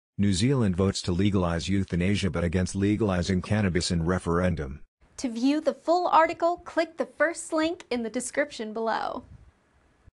New Zealand votes to legalise euthanasia but against legalising cannabis in referendum
New Zealanders have voted to legalise euthanasia for those with a terminal illness, in a landslide victory for campaigners who say anyone suffering extreme pain should be given a choice over how and when to bring their life to a close. The decision on whether to legalise euthanasia appeared as a referendum question on the 17 October general election ballot paper, alongside a second referendum question on whether to legalise cannabis  which did not succeed. The results of the euthanasia referendum are binding and will see the act come into effect 12 months from the final results  on 6 November 2021.  Assisted dying will be administered by the Ministry of Health. Preliminary results announced today by the electoral commission saw 65. 2% of eligible voters ticked yes to legalising euthanasia, with 33. 8% ticking no. Only 46. 1 % of New Zealanders voted to legalise cannabis, while 53. 1% voted no, meaning the legislation would not proceed to parliament. For years support for euthanasia has hovered around the 60-70% mark in polls, with widespread backing across the political spectrum, from prime minister Jacinda Ardern to opposition leader Judith Collins. The vote makes New Zealand only the seventh country in the world to legalise assisted dying, and it was a momentous day for the country, said campaigner Mary Panko. Its now clear what we have known for decades that Kiwis want, and have always wanted, the right to die on their own terms, said Panko. One day New Zealanders will shake their heads in amazement that the basic human right to say no to intolerable suffering ever had to be debated in this country  now because of the passing of this Act our lives as well as our deaths will be immeasurably better. The referendum follows the passing of the End of Life Choice Act in parliament in 2019.  The act would allow those with a terminal illness to apply to terminate their life.  Although it passed it was only